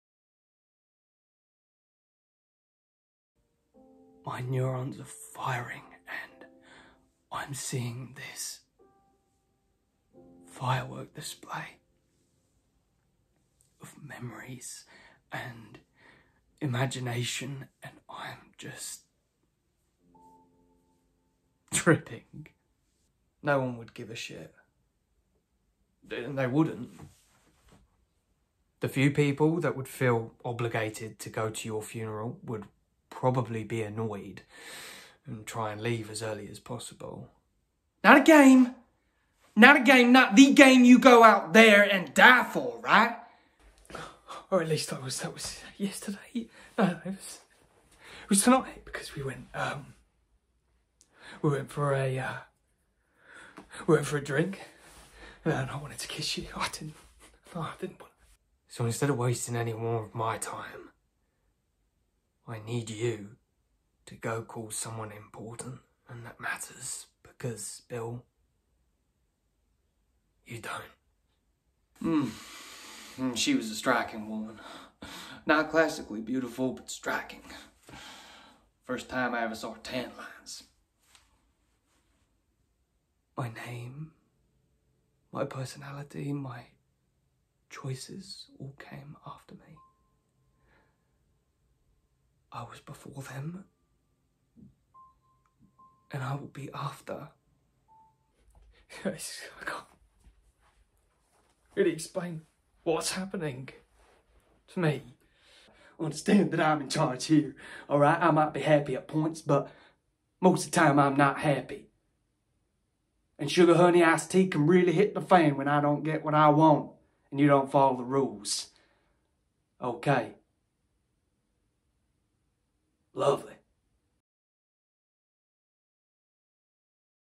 Tom Metcalf Monologue Showreel 2022
A compilation of monologues due to change of appearance. Filmed January 2022.